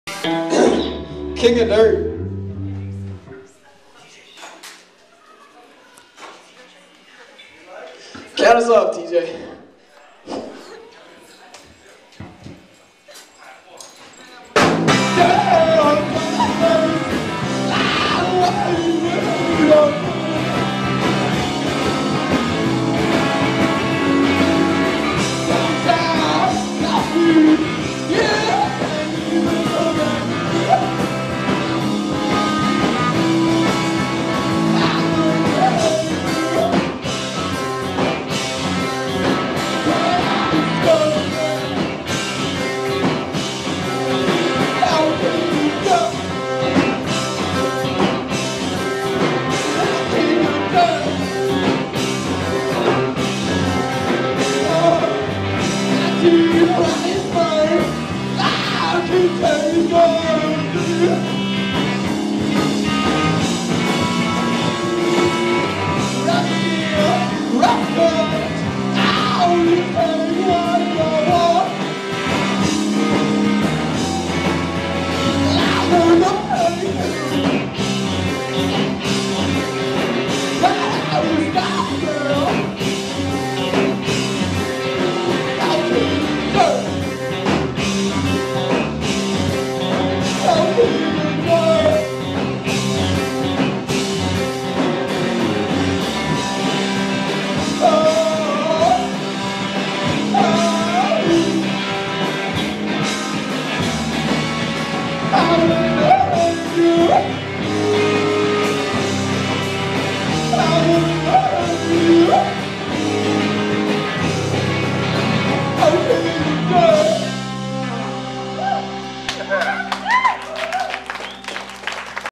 Random 2013 Veracity
A random early 2013 Veracity show. If anybody knows who the band is, leave me a message. I like to give appropriate credit.